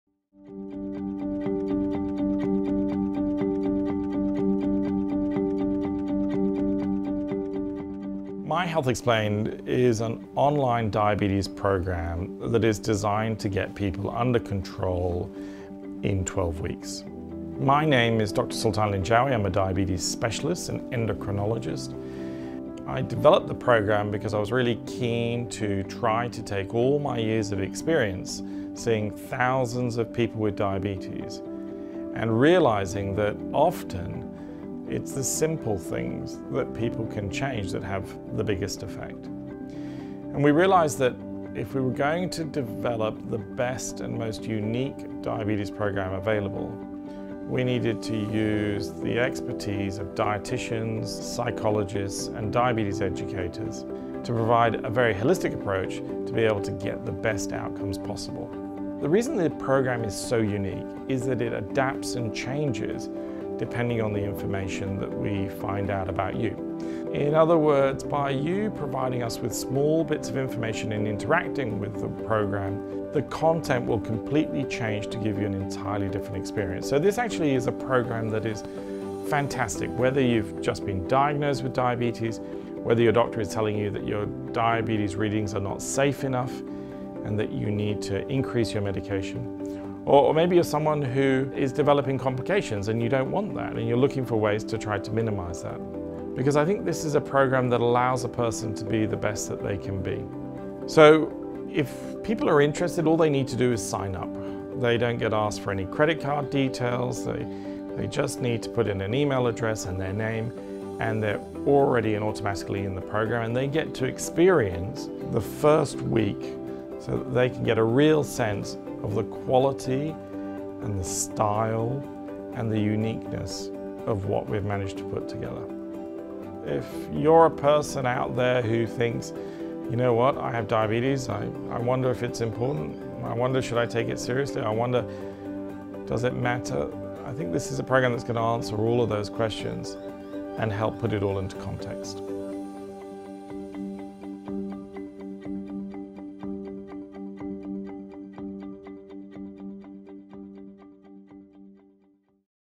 What is My Health Explained and why will it help?
MyHealthExplained.com is a 12 week online diabetes education program to help people with diabetes get on track with their diabetes.

Created by endocrinologist and diabetes medical specialist Dr Sultan Linjawi with the help of dieticians, diabetes educators and psychologists, MHE empowers people with diabetes to better understand their own health so they are navigate, direct and be a partner in their own health care.

My Health Explained is an online diabetes program that is designed to get people under control in 12 weeks.
My name is Dr Sultan Linjawi, I am a Diabetes Specialist, an Endocrinologist.
I developed the program because I was really keen to try to take all of my years of experience seeing thousands of people with diabetes and realising that often it is the simple things that people can change that have the biggest effect.
And we realised that if we were going to develop the best and most unique diabetes program available, we needed to use the expertise of dietitians, psychologists and diabetes educators to provide a very holistic approach to be able to get the best outcomes possible.
The reason the program is so unique, is that it adapts and changes depending on the information that we find out about you.
In other words, by you providing us with small bits of information and interacting with the program, the content will completely change to give you an entirely different experience.
So this actually is a program that is fantastic.
Whether you have just been diagnosed with diabetes whether your doctor is telling you that your diabetes readings are not safe enough and that you need to increase your medication, or maybe you are someone who is developing complications and you do not want that, and you are looking for ways to try to minimise that.
Because I think this is a program that allows a person to be the best that they can be.
So, if people are interested, all they need to do is sign up. They do not get asked for any credit card details, they just need to put in an email address and their name and they are already automatically in the program and they get to experience the first week so that they can get a real sense of the quality and the style and the uniqueness of what we have managed to put together If you are a person out there who thinks "You know what, I have diabetes, I wonder if it is important? I wonder if I should take it seriously? I wonder does it matter?" I think this is a program that is going to answer all of those questions and help put it all into context.